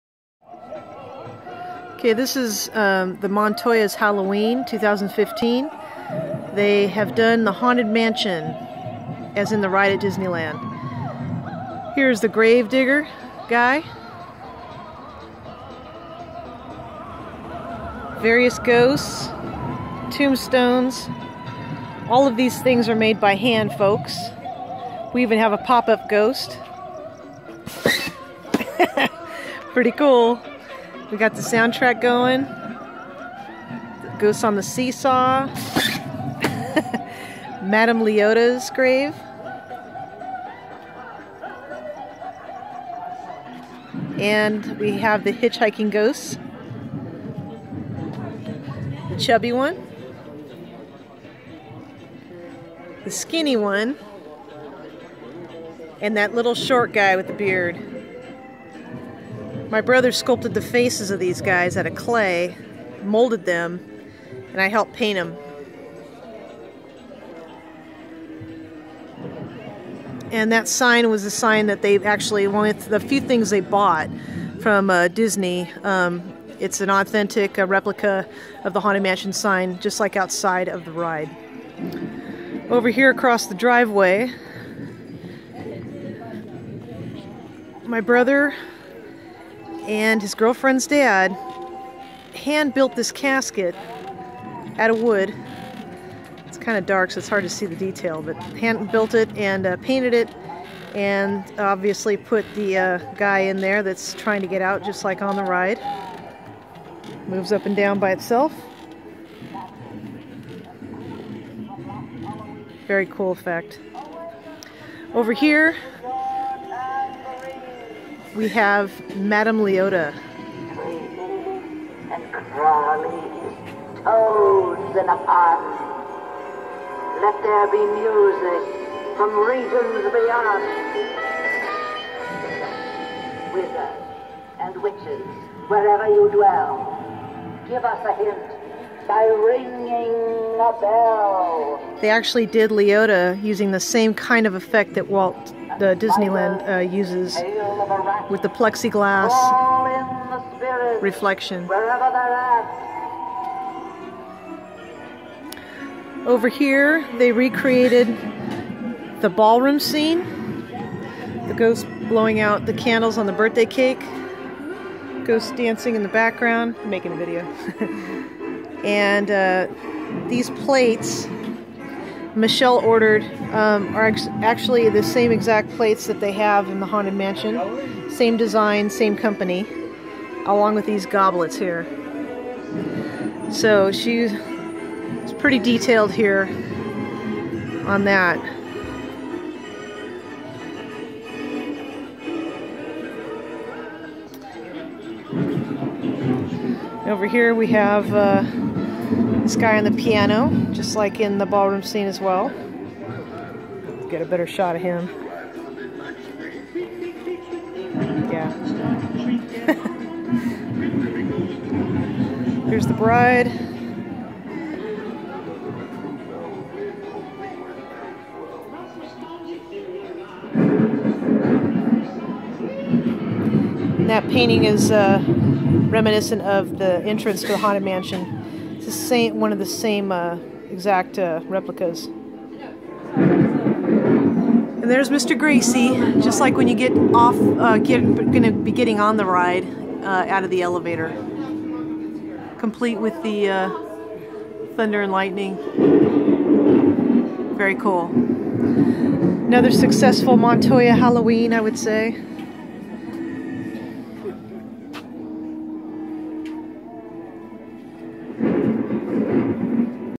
Amazing Disneyland Haunted Mansion Display
This amazing display was created, mostly by hand, by my brother and the Montoya family  (I helped a little too).  The Montoyas live in Rancho Penasquitos, CA and every year they do a different theme for Halloween,  only to be displayed on Halloween night.  Weeks of preparation and hard work go in to this, but it is a labor of love.  Hope you enjoy it!